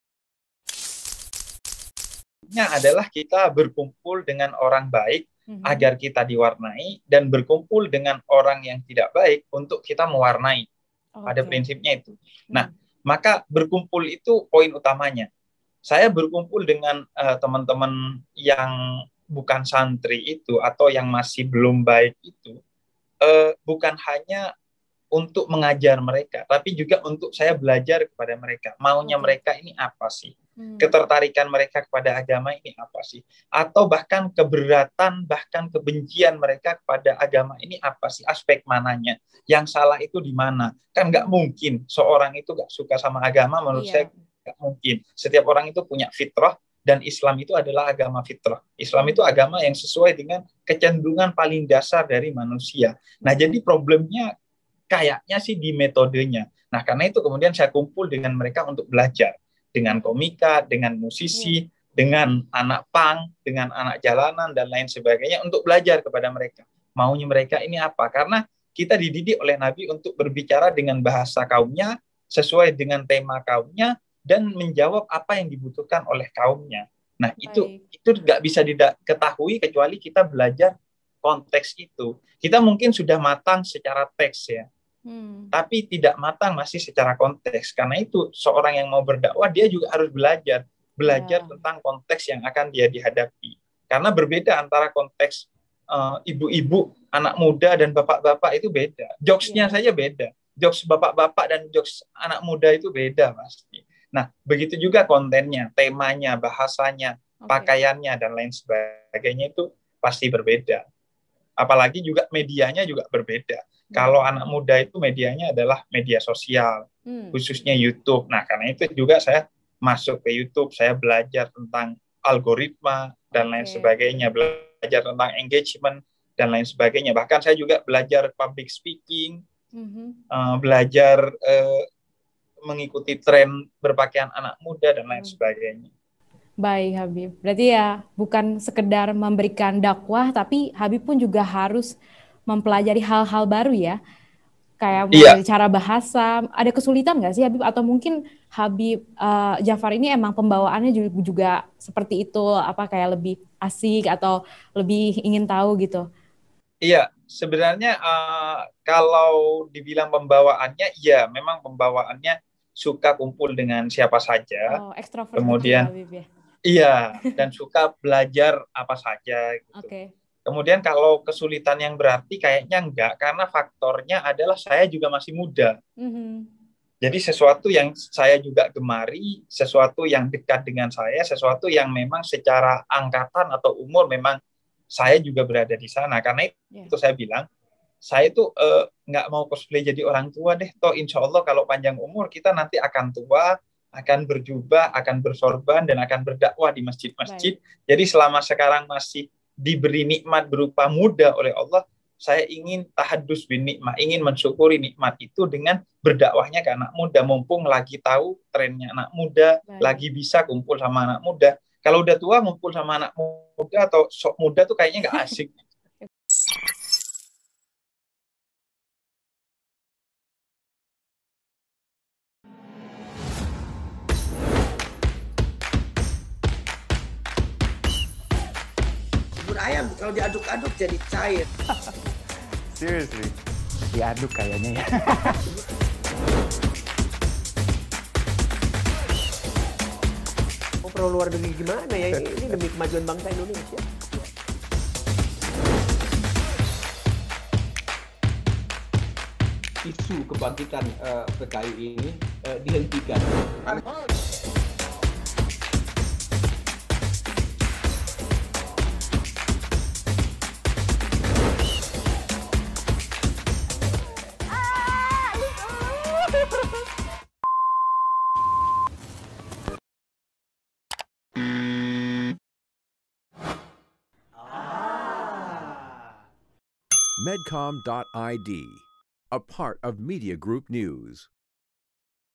Cara Habib Husein Ja'far Sebarkan Ilmu Agama ke Generasi Muda
Salah satu pendakwah yang kini digandrungi millennial adalah Husein Ja’far Al Hadar. Lelaki yang karib disapa Habib Husein ini dikenal sebagai da’i yang mengusung prinsip Islam cinta. Alih-alih berdakwah dengan lantunan ayat Alquran dan hadis, Husein lebih sering berdakwah melalui canda dan nada.

Habib Ja'far sering berkolaborasi dengan para content creator dalam memperkenalkan Islam. Ia memiliki cara tersendiri untuk memperkenalkan agama islam masuk kedalam lingkungan generasi anak muda. Habib Ja'far tidak ingin menampilkan diri bak pengkhotbah dengan jubah dan balutan sorban, sehingga orang-orang segan dengannya. Dalam menyampaikan pesan-pesan Allah SWT, Husein ingin menampilkan diri selayaknya seorang sahabat yang mengajak temannya untuk berbuat baik.